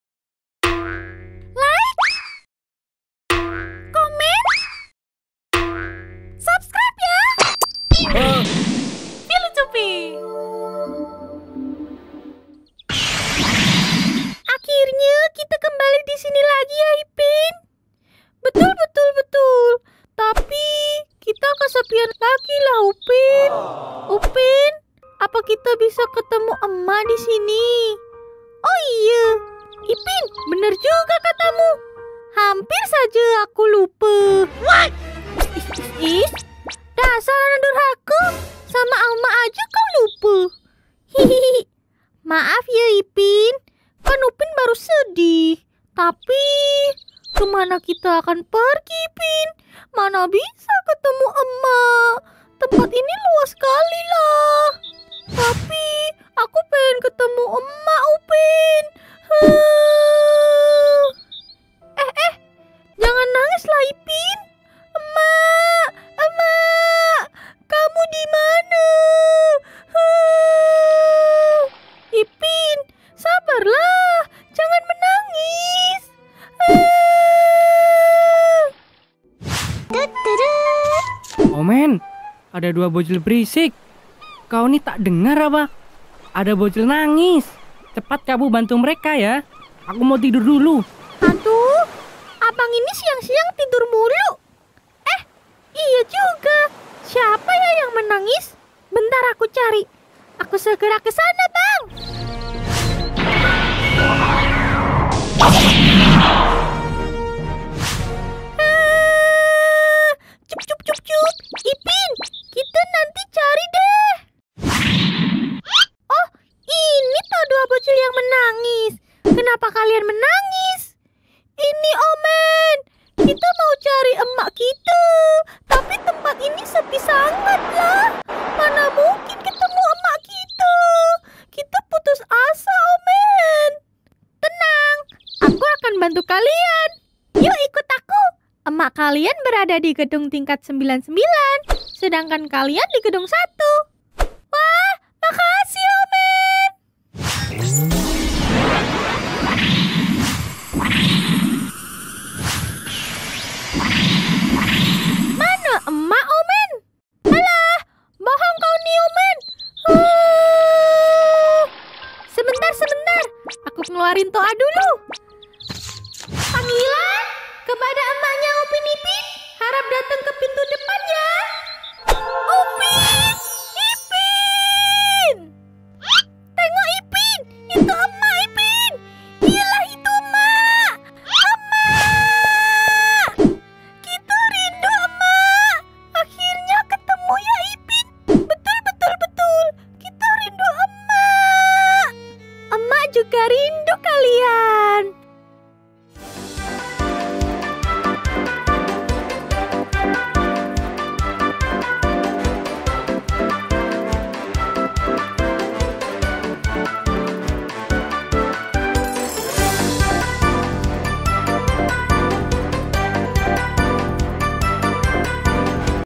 Upin Ipin Bertemu Emak
Ketika upin dan ipin kesepian di langit mereka bingung kenapa sepi sekali akhirnya ipin berfikir bisa ketemu dengan emaknya ,tetapi mereka putus asa karena tidak tau harus mencari kemana dan akhirnya meminta tolong omen untuk membantu bertemu dengan emaknya..

FILUCUPI adalah animasi yang mengisahkan kejadian lucu dalam kehidupan sehari-hari. Dikemas dalam bentuk animasi kartun lucu yang menghibur.